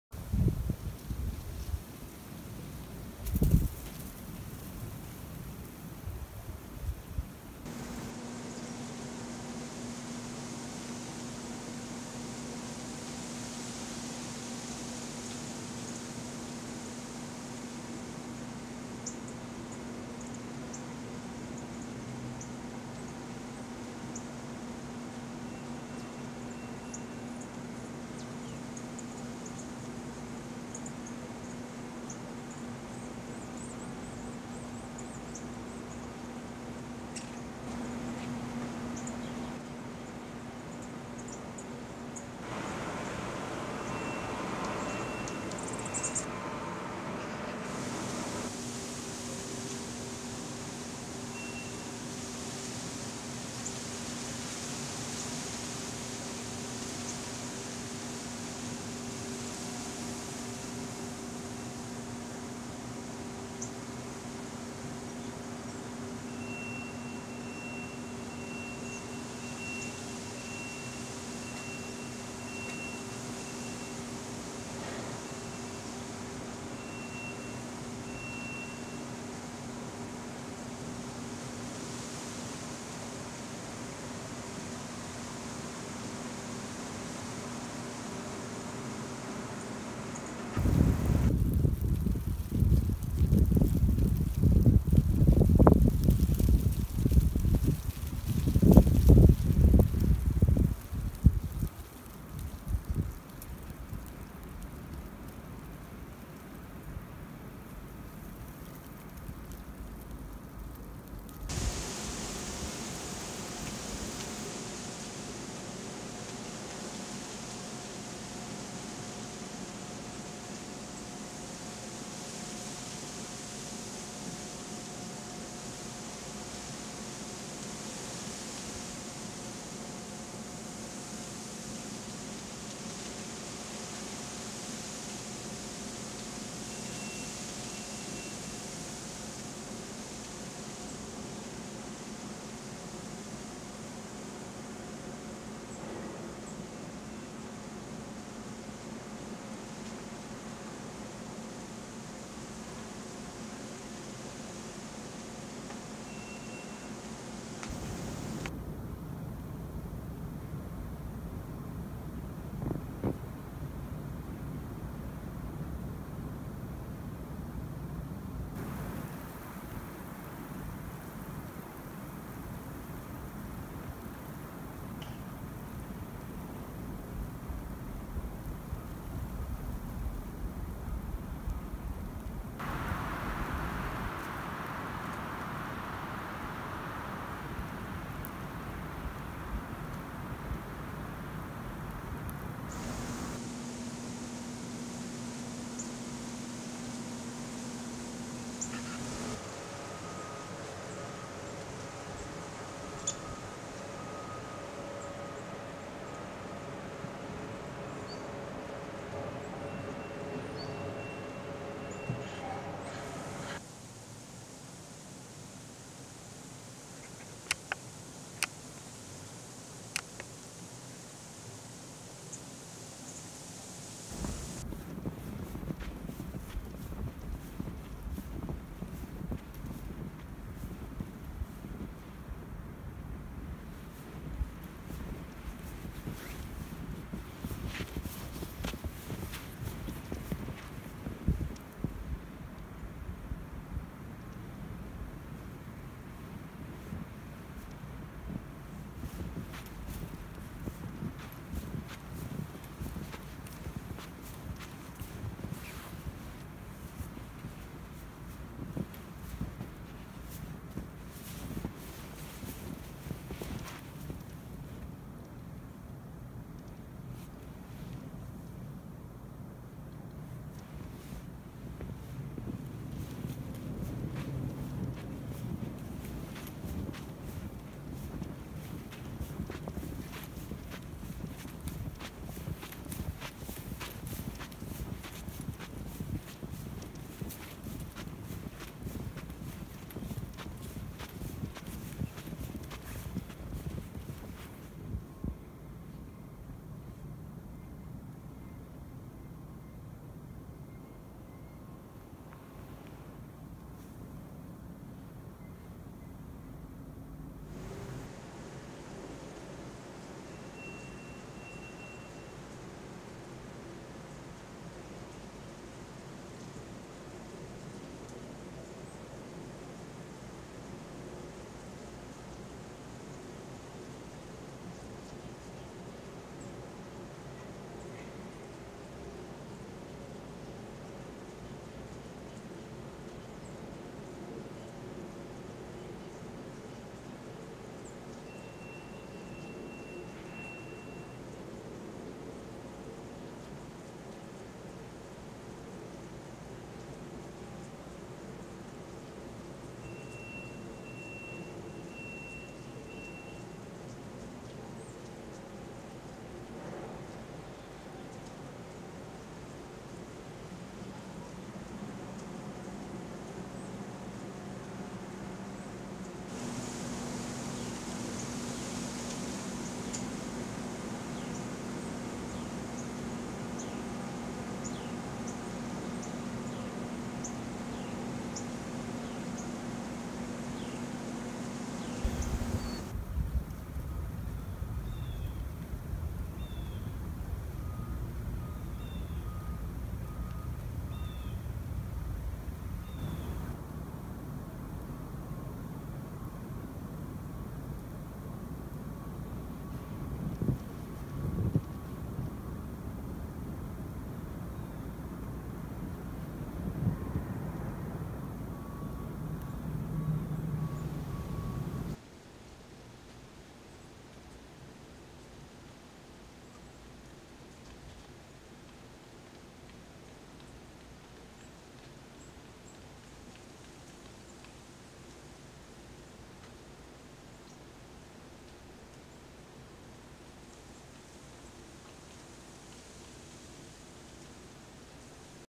Snow December Birds
00:07, 3:18 Tufted Titmouse (Baeolophus bicolor)
00:38, 3:33, 5:07 Dark-eyed Junco (Junco hyemalis)
01:53 Downy Woodpecker (Dryobates pubescens)
03:12 Black-capped Chickadee (Poecile atricapillus)
04:53 Two dimensional Pine
nikon d3500 and tamron 500mm f8
iPhone